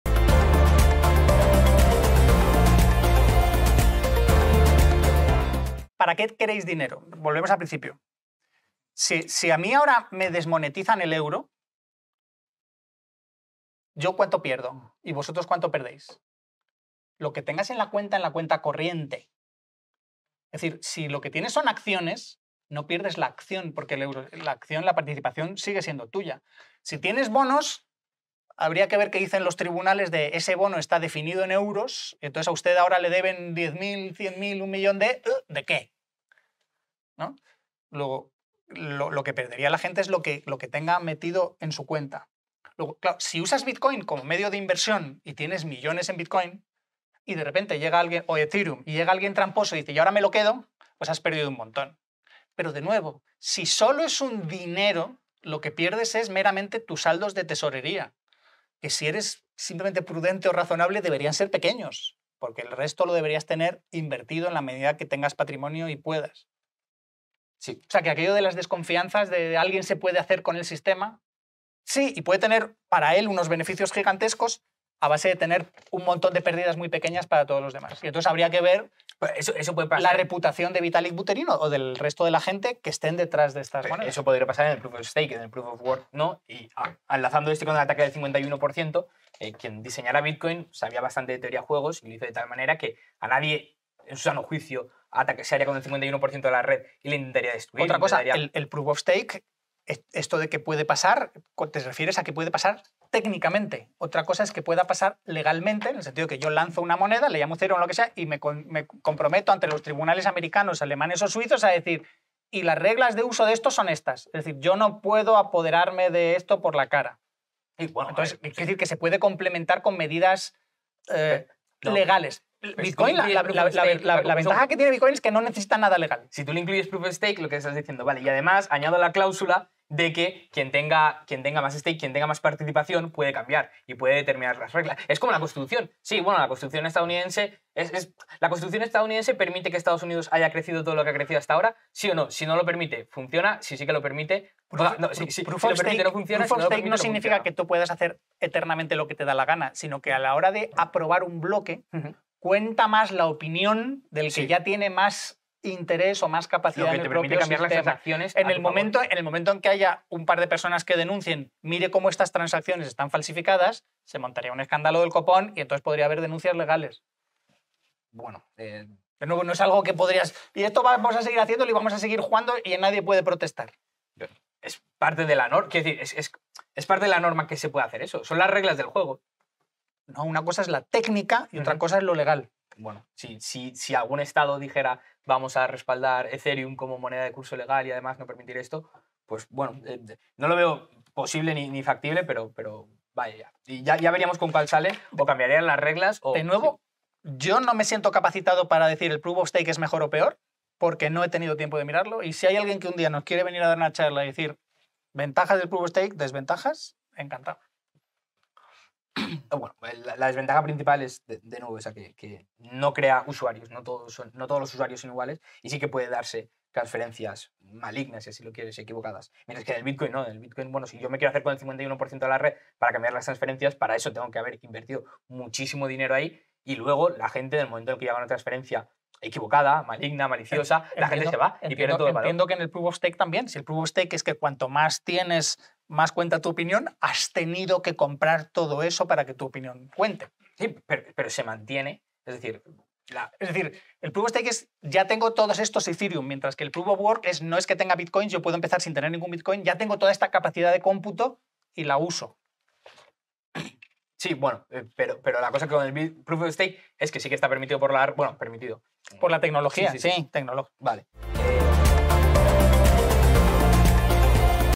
El DEBATE SOBRE BITCOIN. ¿Para qué queréis el DINERO?
🔍 Si estás interesado en aumentar tu conocimiento sobre Bitcoin.
➡️ Ten tu propio pensamiento. Y así determinar con argumento si estas a favor de Capella o de Blasco.👇

Capella nos pregunta, para qué queréis el dinero.

En la discusión sobre el propósito del dinero, Paco Capella plantea la pregunta fundamental de para qué se necesita realmente el dinero. Explica que, en esencia, el dinero debe ser un medio de intercambio eficiente, un depósito de valor confiable y una unidad de cuenta estable para que las personas lo usen de manera generalizada. Argumenta que si un sistema monetario no cumple con estas funciones básicas de manera efectiva, como puede ser el caso de Bitcoin en su forma actual, entonces su utilidad se ve comprometida. Capella enfatiza que la estabilidad y la liquidez son cruciales para que el dinero sea funcional en la vida cotidiana y en la economía en general, sugiriendo que sin estas características, la aceptación de Bitcoin como dinero será limitada.